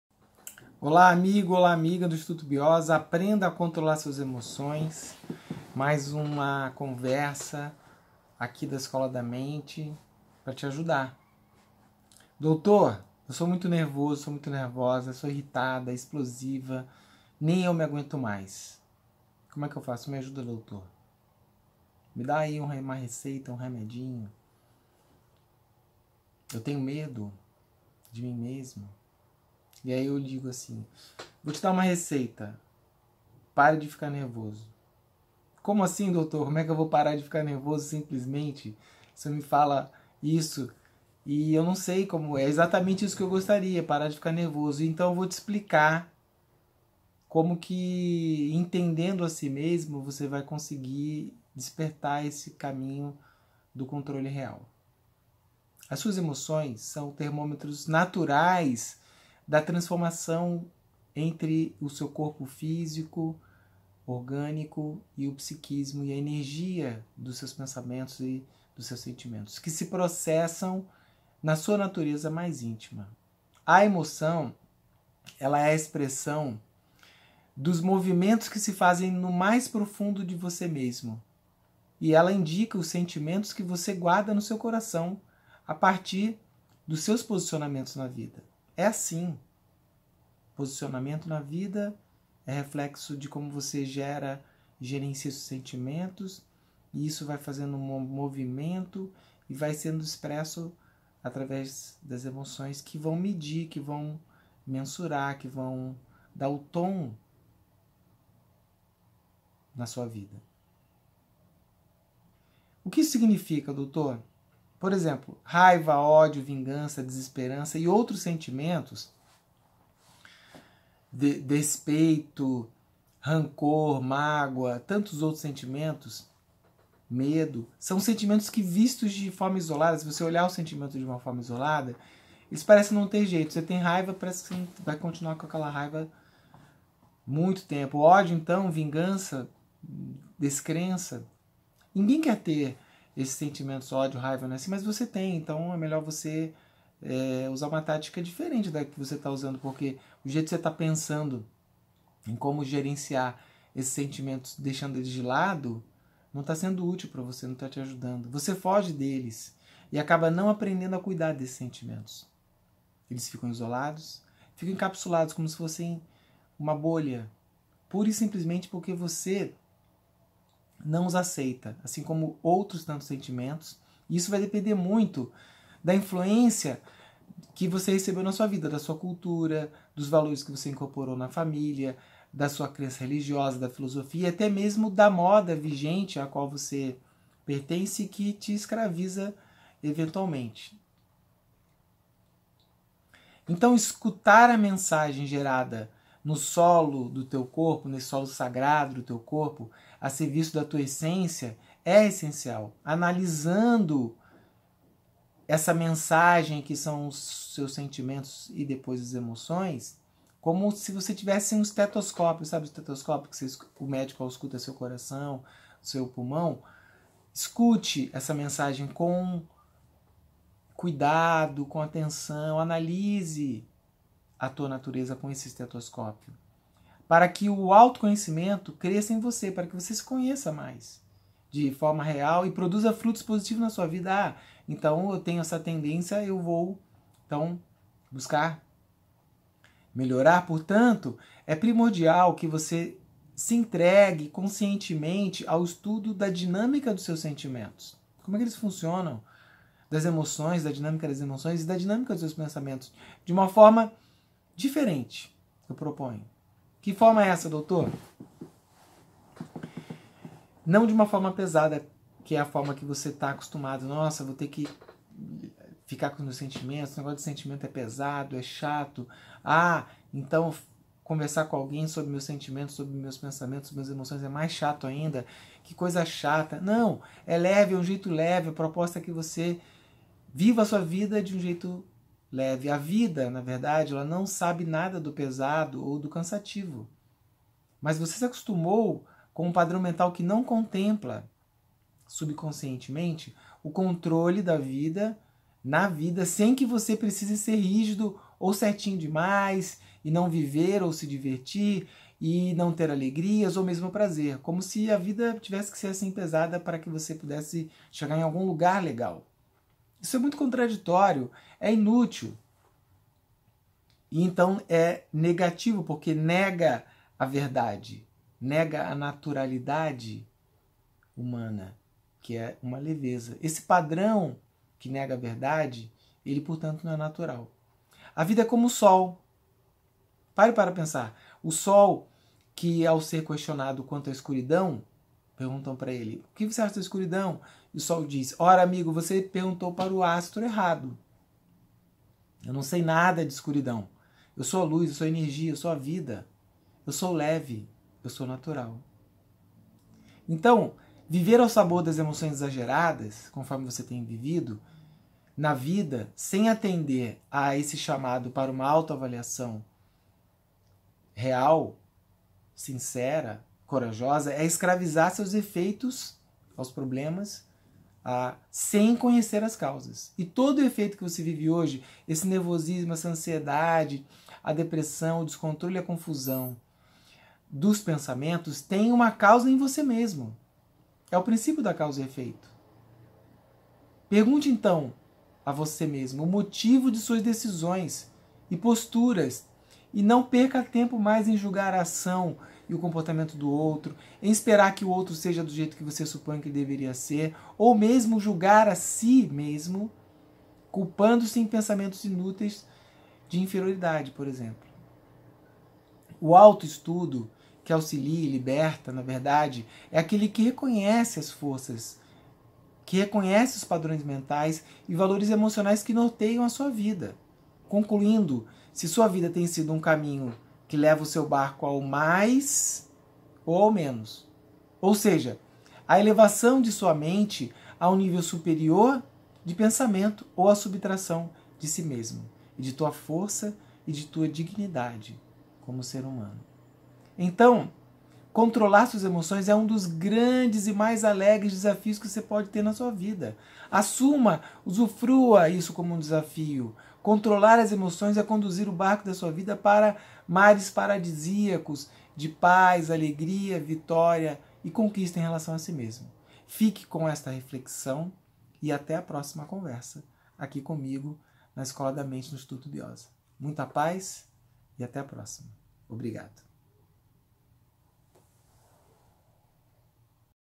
APRENDA A CONTROLAR AS SUAS EMOÇÕES _ "sou nervoso, irritado e explosivo...nem eu me aguento mais !
As suas emoções são o termômetro das transformações físicas, psíquicas e energéticas que se passam em seu íntimo. Nessa conversa o Dr Romulo Bioza te conduz ao desenvolvimento de uma escuta íntima leve e que aceita a sua verdade e naturalidade!